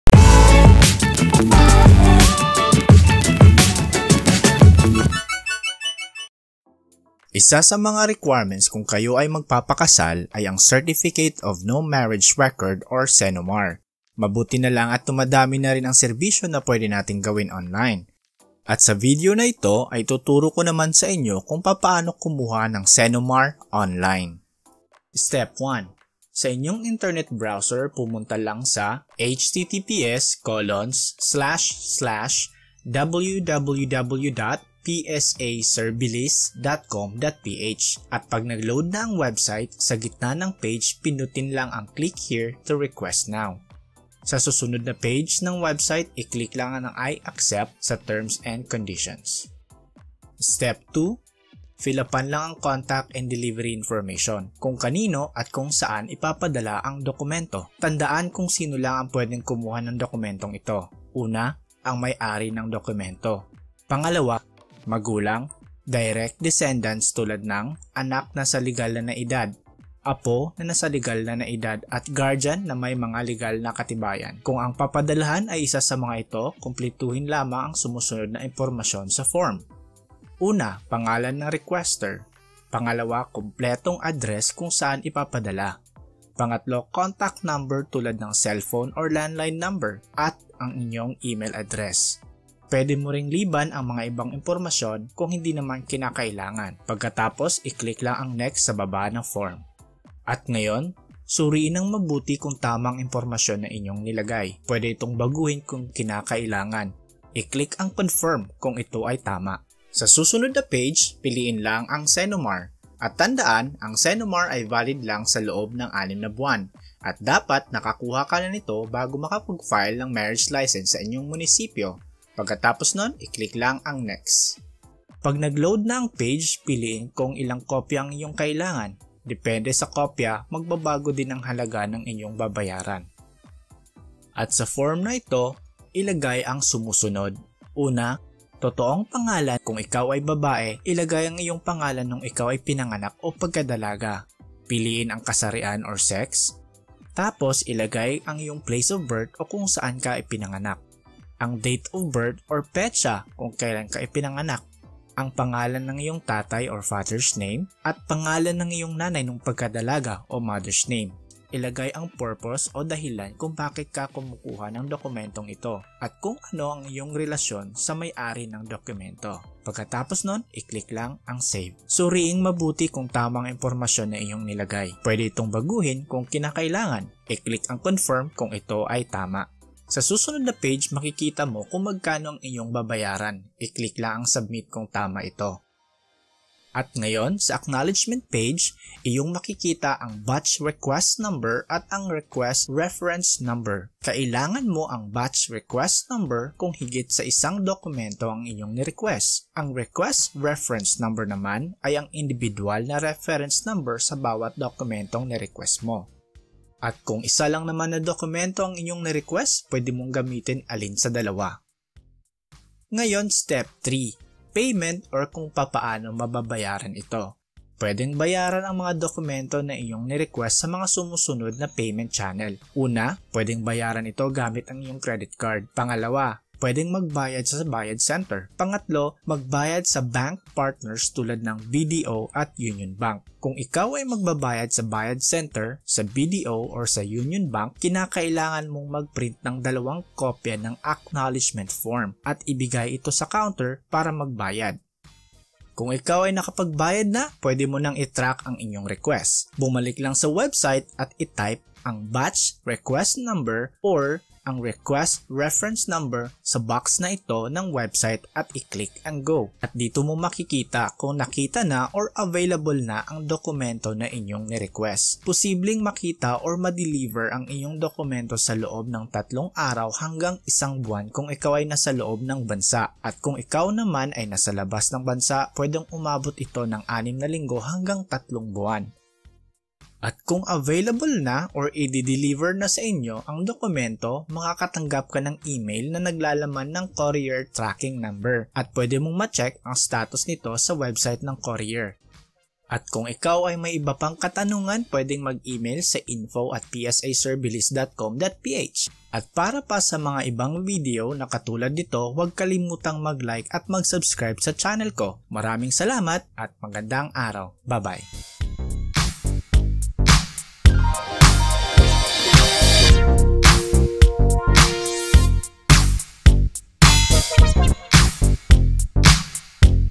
PSA CENOMAR - How to Request Online
Isa sa mga requirements kung kau ay magpapakasal, ay ang  Certificate of No Marriage Record or CENOMAR
Mabuti nalang dumadami nadin ang mga serbisyo na pede nateng gawin online.

Sa video na ito ituturo ko sa ino kung papaano kumuha ng CENOMAR Online
Ang CENOMAR ay valid lang sa loob ng anim na buwan.
At dapat nakakuha kana nito bago ka makapag-file ng Marriage license sa  Munisipyo.